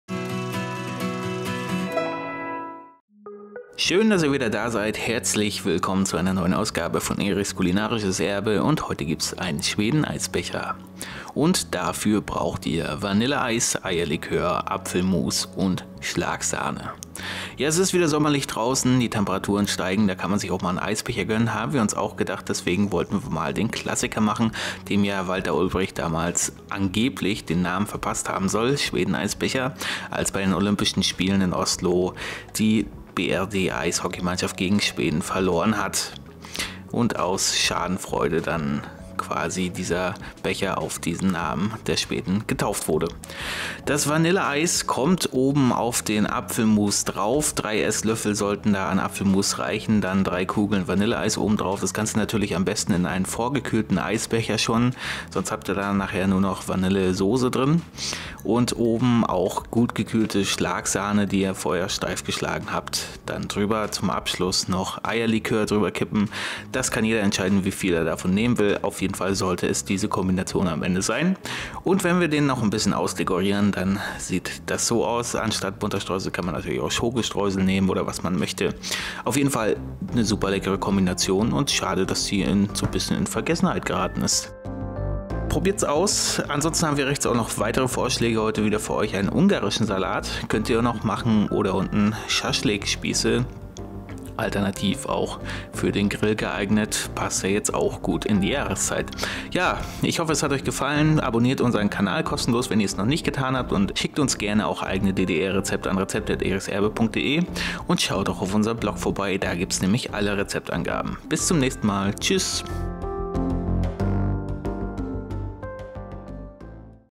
Walter Ulbrichts Lieblings-Eisbecher selber machen: DDR Rezept für Schwedenbecher
So soll der Schwedenbecher (auch Schwedeneisbecher) mit Apfelmus und Eierlikör aus diesem DDR Rezept zu seinem Namen gekommen sein: 1952 verlor die BRD-Eishockeymannschaft bei den Olympischen Winterspielen in Oslo gegen die Schweden mit 3 : 7. Aus Schadenfreude über die Niederlage des Klassenfeindes soll Walter Ulbricht die bevorzugte Eiskreation nach diesem DDR Rezept fortan Schwedenbecher genannt haben. Alle Angaben zum DDR Rezept für Schwedeneisbecher findest du und in unseren Kochbüchern